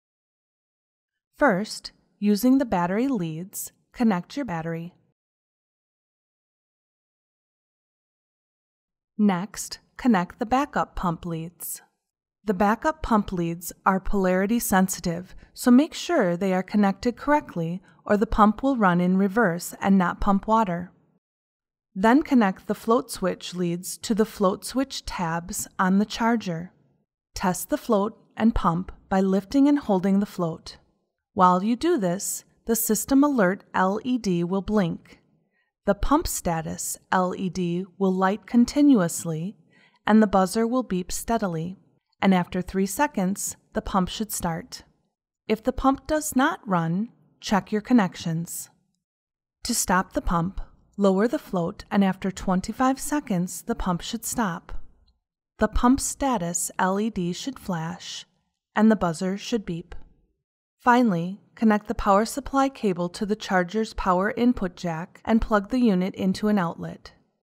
Wiring the Pentair Battery Backup Unit
This video demonstartes the quick and easy steps to wiring the Pentair Battery Backup Unit.